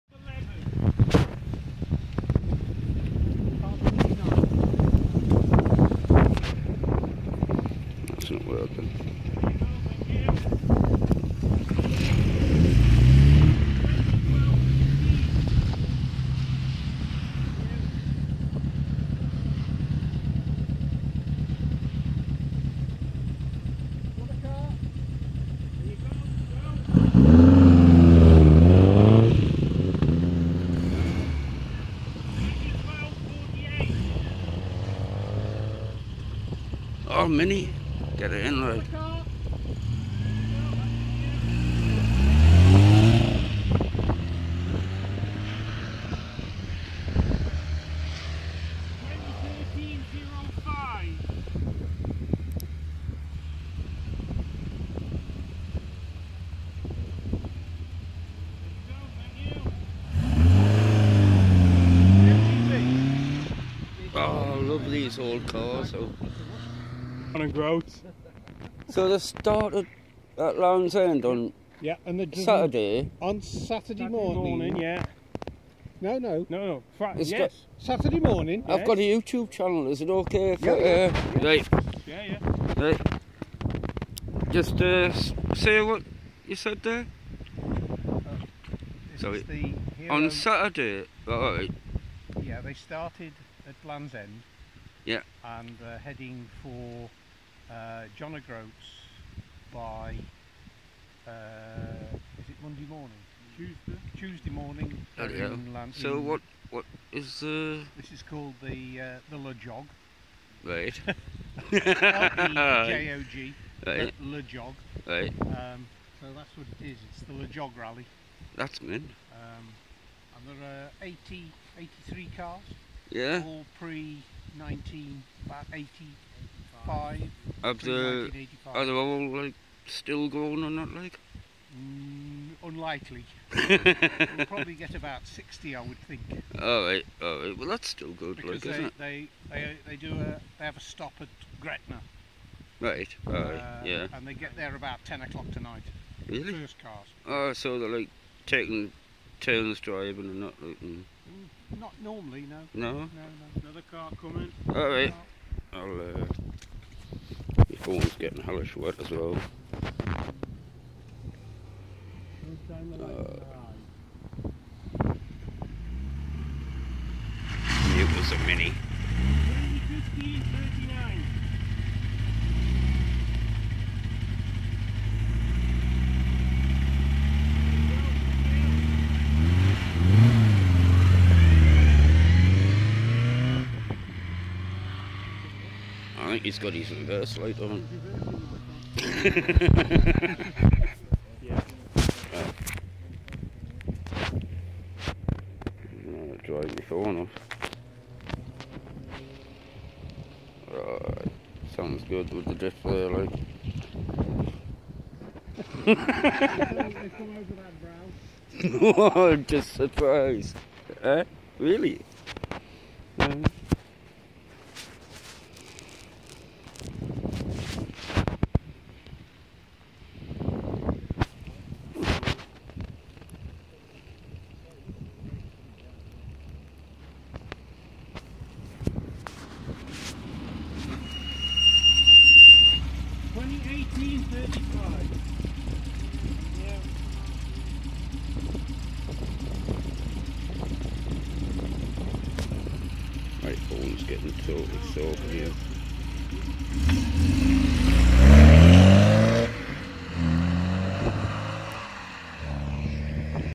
Lands end to John o groats vintage MG rally from Saturday morning to Tuesday!
Some lovely old cars rallying past my road end.😉👍🍻